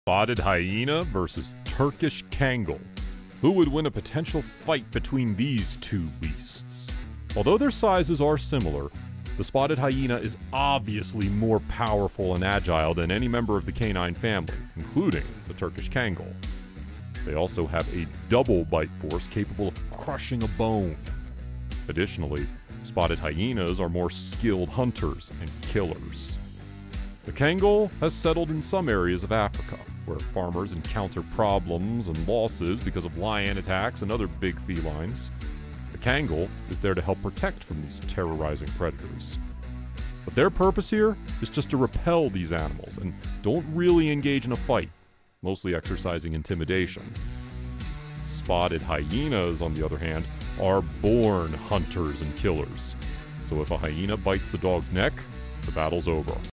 Turkish Kangal Vs Spotted Hyena - The Ultimate Verdict
Spotted hyena VS Turkish Kangal
Who would win a potential fight between these two beasts

Imagine a fight between the Turkish Kangal, one of the strongest of guardian dog breeds, and Africa’s best predators and lion rivals--the spotted hyena.

So what are their strengths?